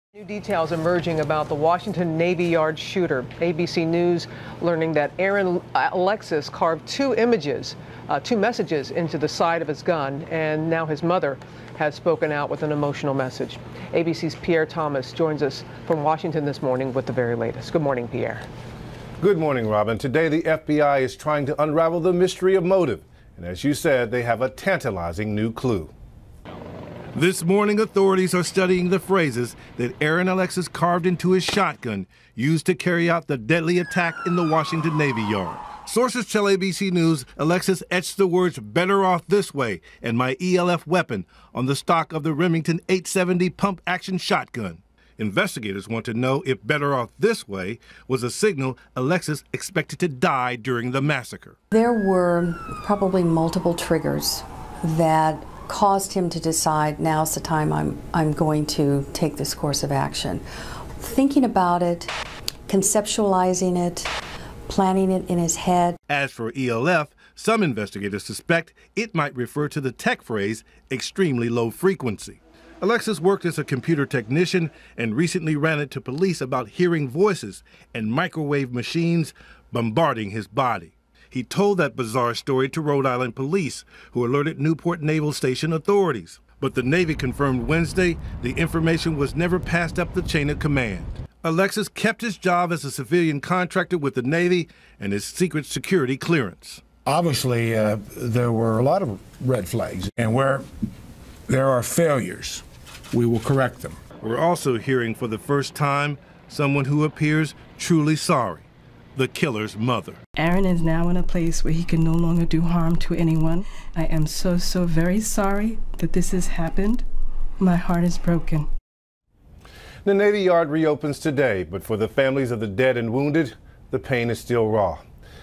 Aaron Alexis: Targeted Individual / Cobayo psicotronico de entidades caco-alienigenas
Activar subtitulos.
"La naturaleza de la actividad alienigena ha sido diseñada para que no podamos tener ninguna evidencia concreta. No es una casualidad sino que ESTA DISEÑADA DE ESA FORMA."     (Karla Turner)

"Muchas de estas experiencias son creadas para que sospechemos enormemente de nuestro gobierno."    (Karla Turner)

"Tu y otras personas seran activadas para llevar a cabo sus trabajos. No importa lo que  esten haciendo, en donde esten o lo que quieran. Haran sus trabajos."
(Cita de los alienigenas a un "contactado" en el libro "Into the fringe" de Karla Turner)